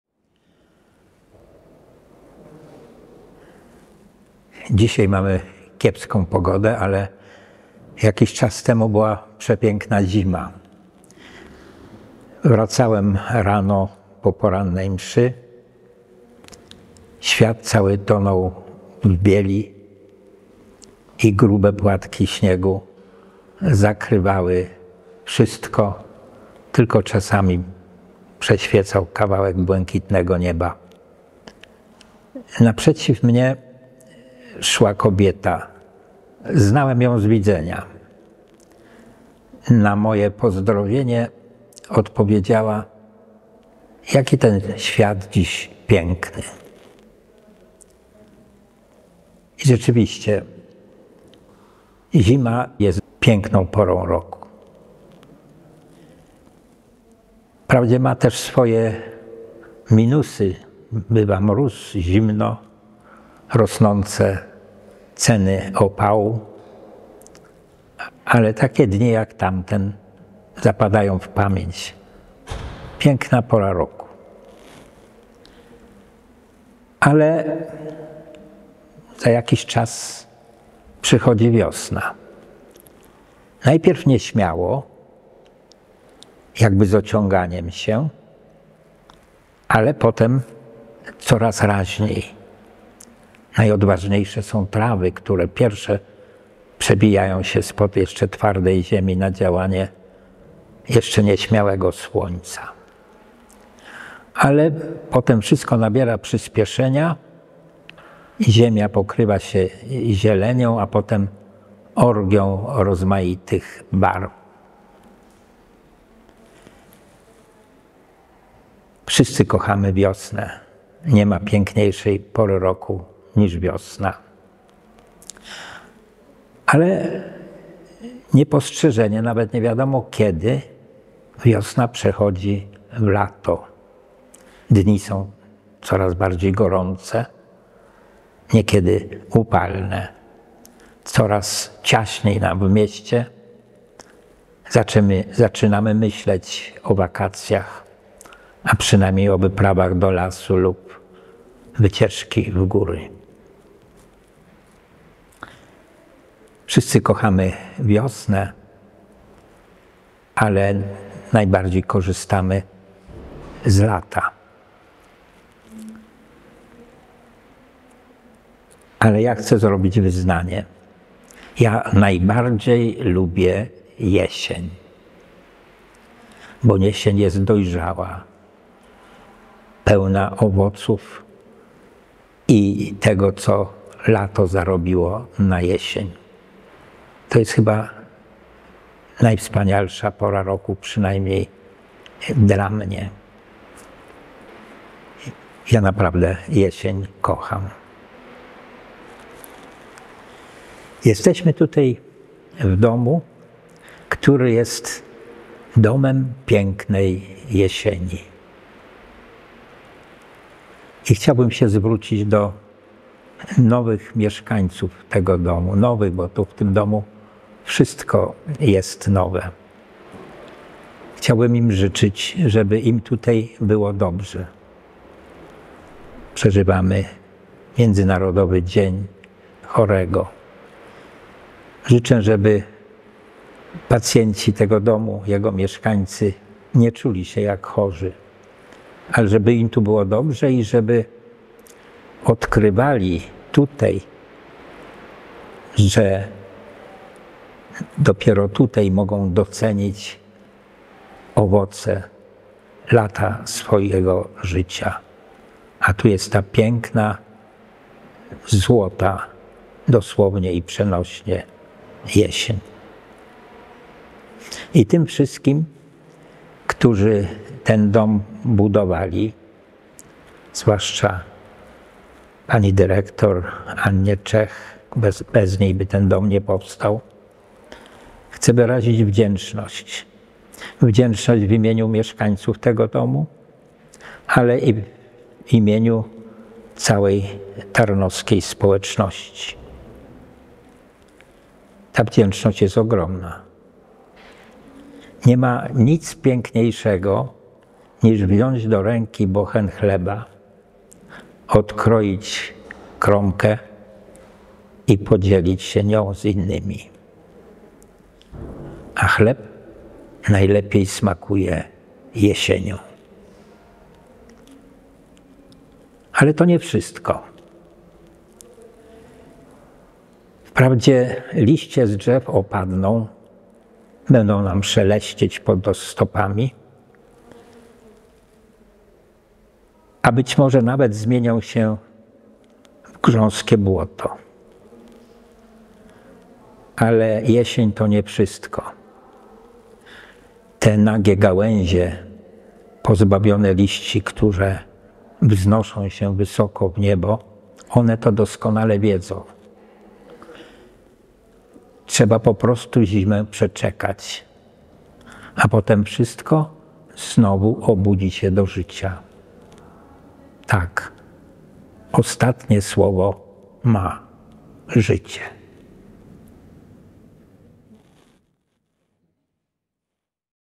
ks. prof. Michał Heller - Refleksja na Światowy Dzień Chorego
W pierwszych dniach funkcjonowania hospicjum Via Spei odwiedził nas ks. prof. Michał Heller, który podzielił się swoimi rozważaniami na zbliżający się Dzień Chorego, obchodzony corocznie 11 lutego.
Ks. prof. Michał Heller to teolog, historyk, filozof i kosmolog, pasjonat nauk ścisłych. Wielokrotnie nagradzany i wyróżniany za pokonywanie barier między nauką a religią oraz za szczególny wkład w rozwój duchowego wymiaru ludzkiego życia.